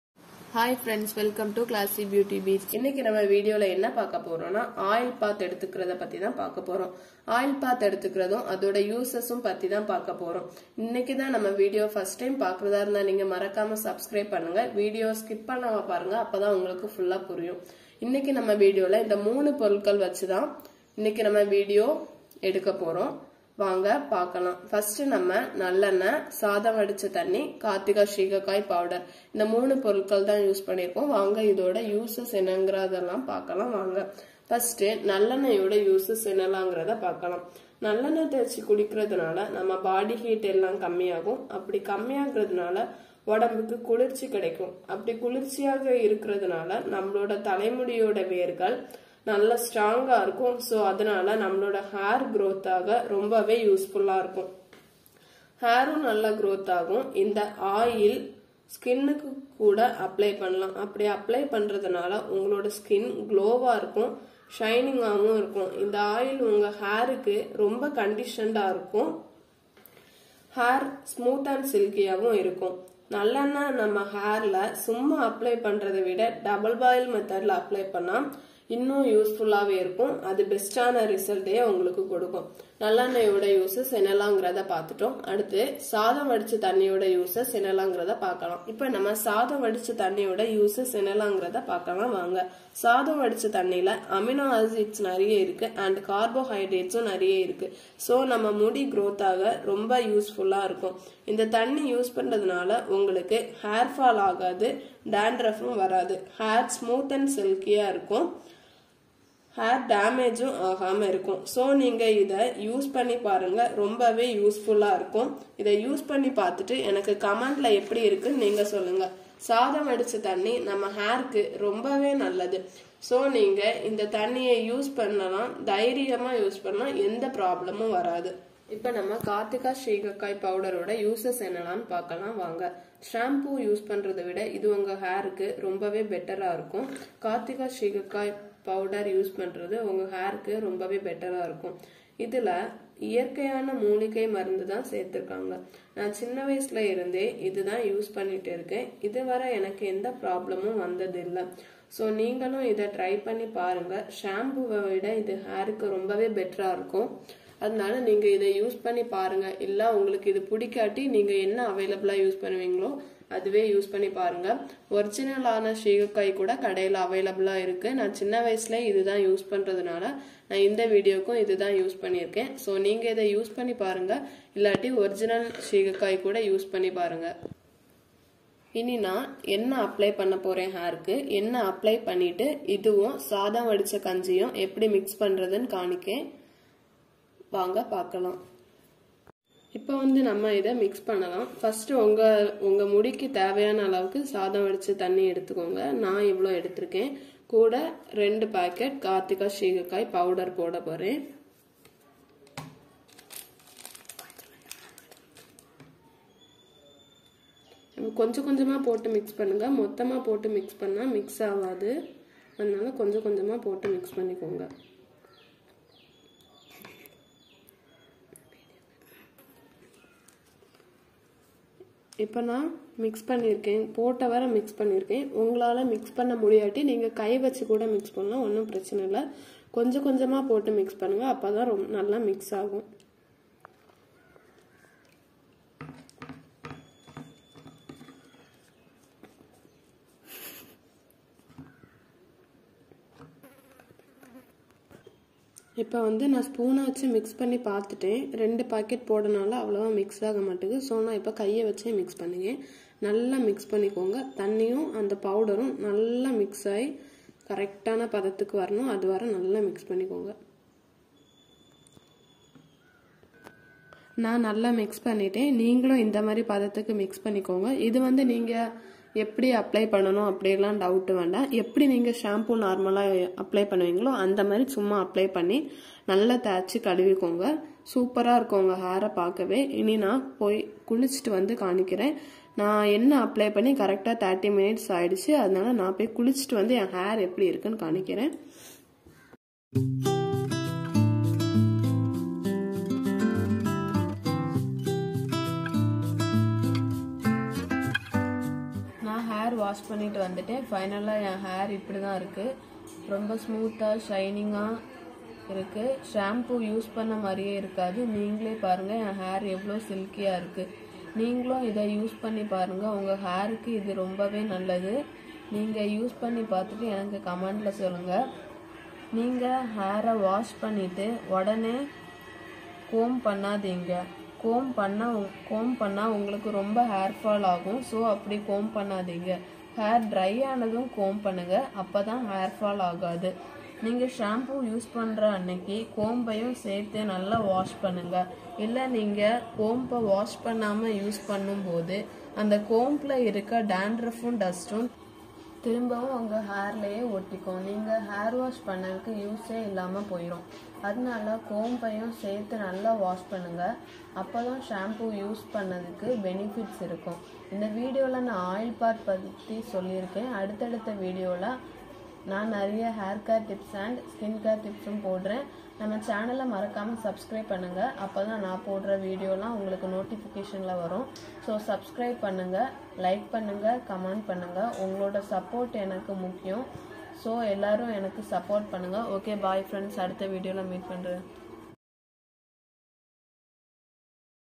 Hair growth oil bath with gingelly oil, rice boiled water, karthika shikakai powder
Oil path

Ingredients👇
Gingelly oil
Rice boiled water
Kathika shikakai powder

Apply oil on 30 mins
Then mix the rice boiled water and kathika shikakai powder mix well and apply our hair and wash  cleanly..
If you have any dought in this video in ask in comment